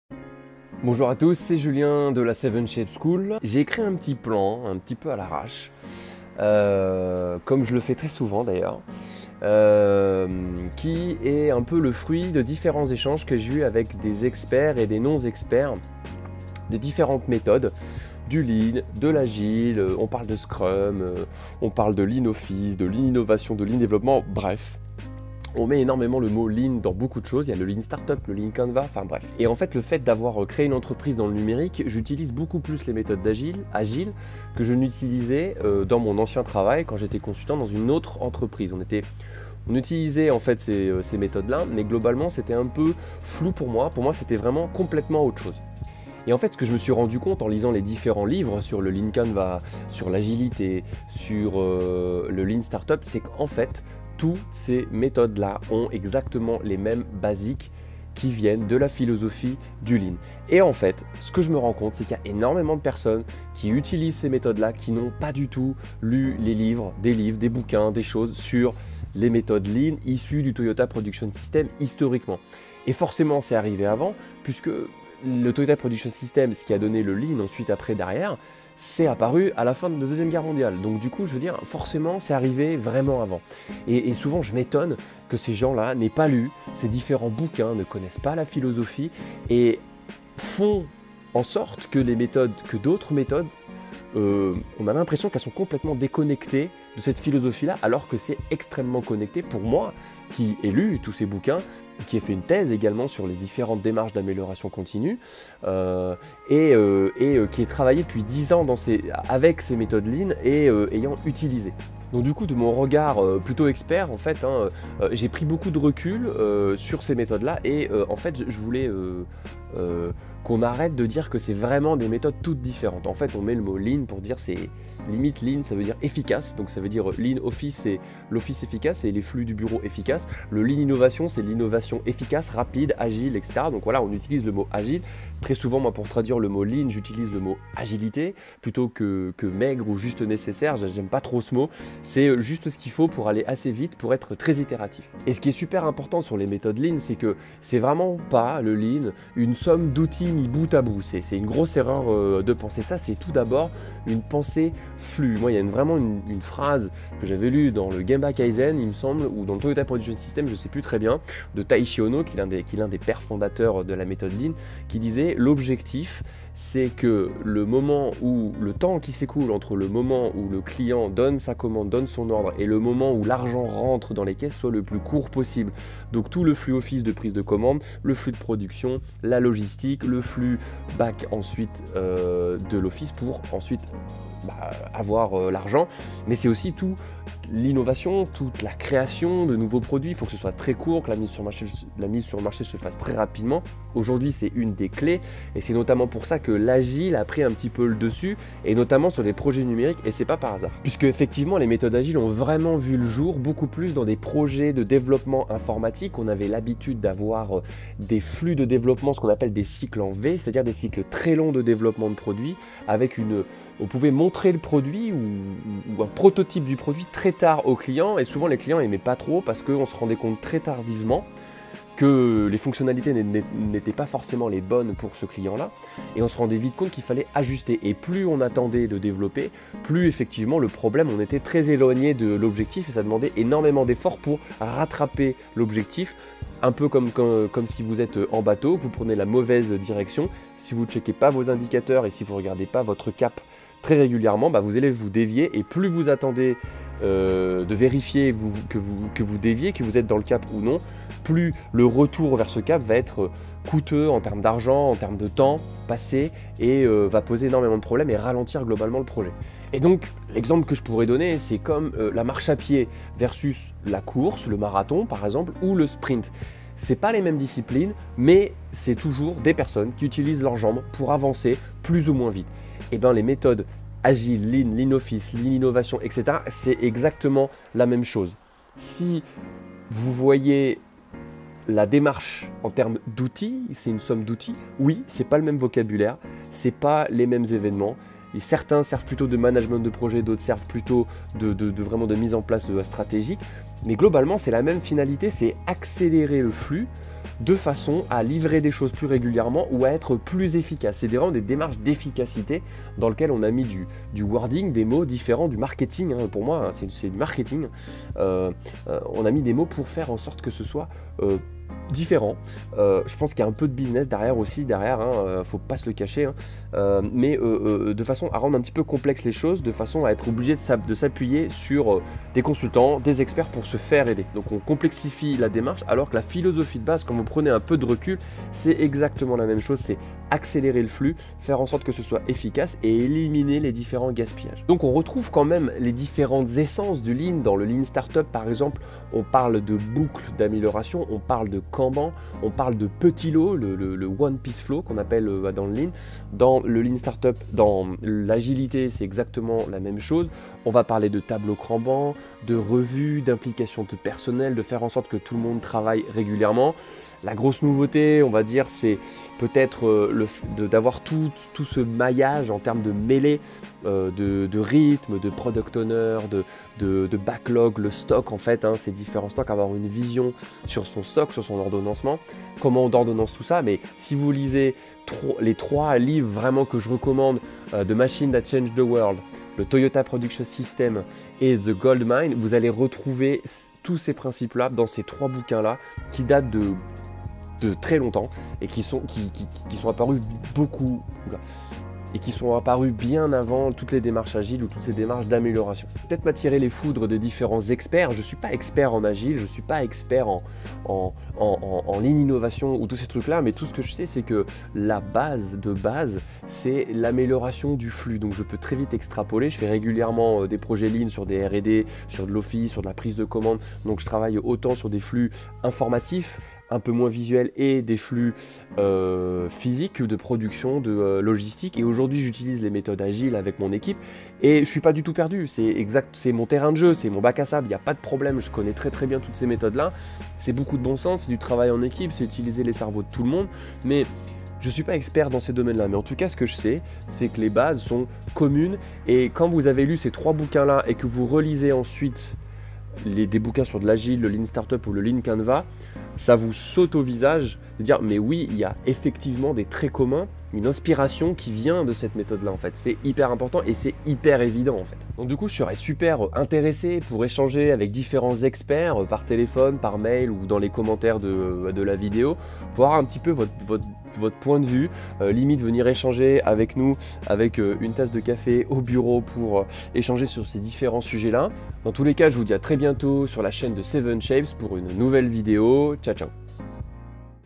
Pourquoi les méthodes Agile, Lean, Lean Startup ont les mêmes bases !!
Je vous partage ici mon point de vue sur les méthodes Lean vs Agile vs Lean Startup vs Lean office etc... Il y en a beaucoup mais elles ont toutes les mêmes bases ! :-)

Julien